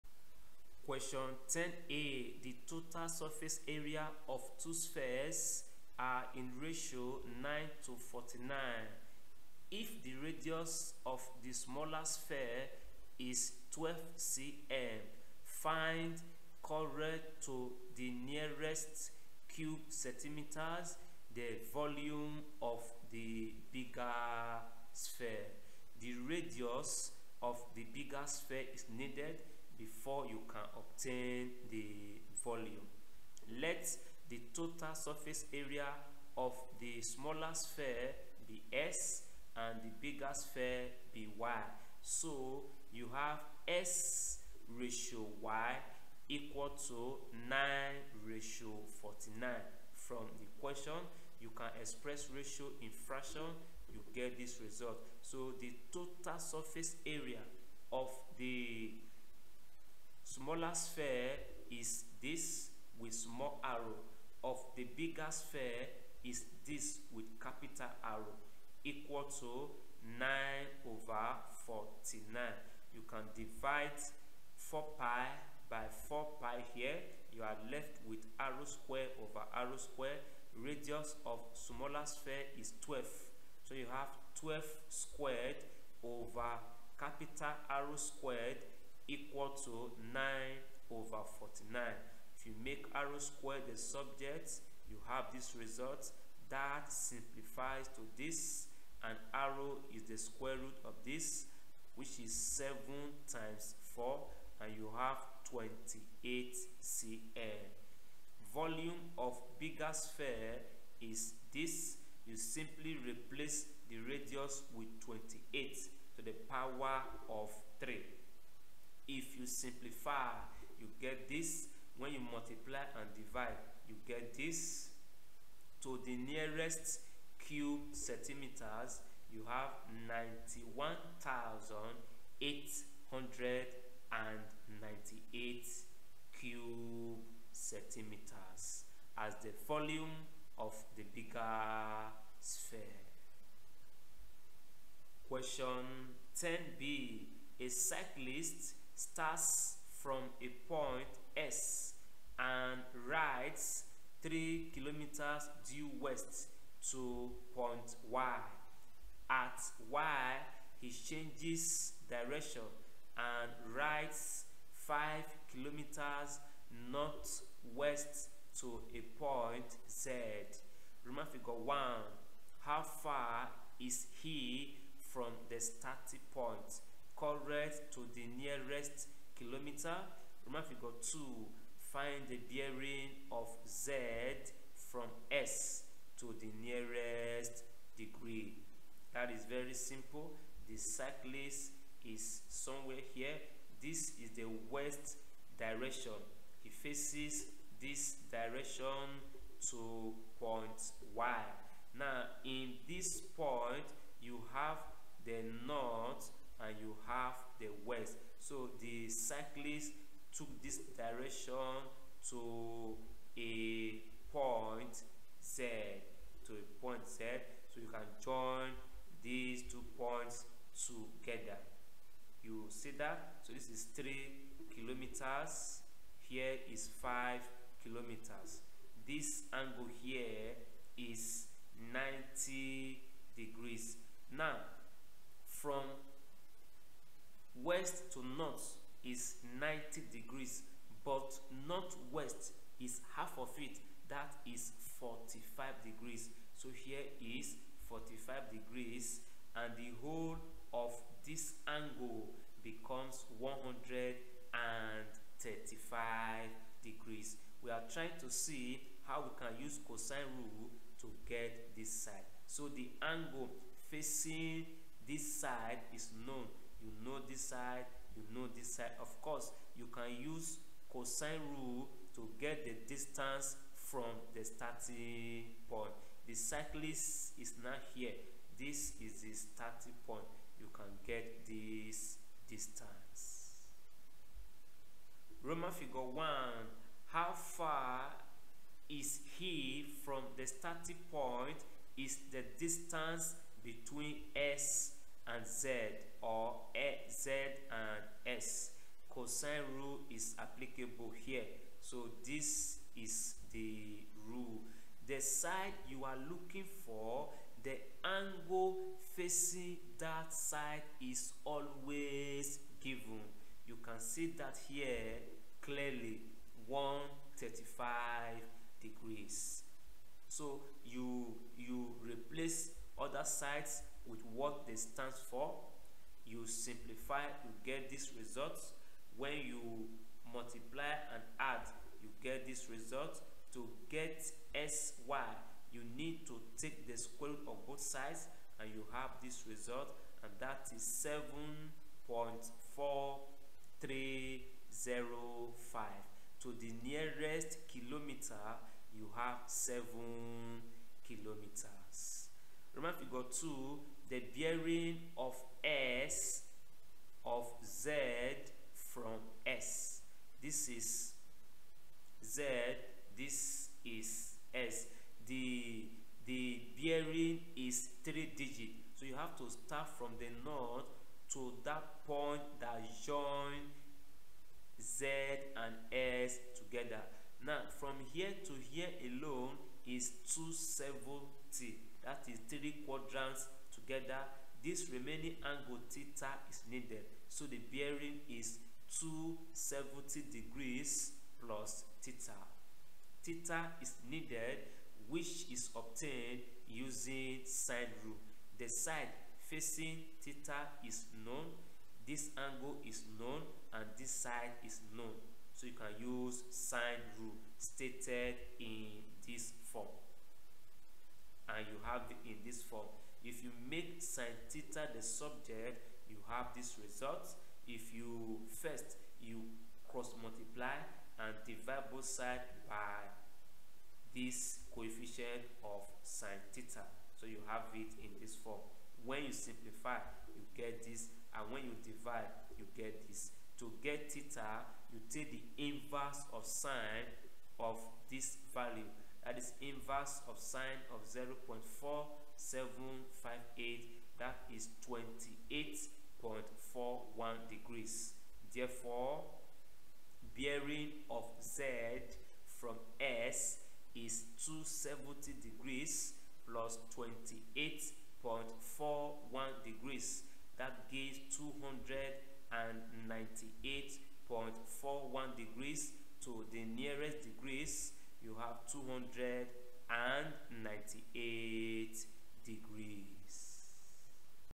WAEC 2011 Mathematics Theory Question 10
This is WAEC 2011 Mathematics Theory Question 10, in this video and others in this series, you will learn the step by analysis on how to solve WAEC 2011 past questions.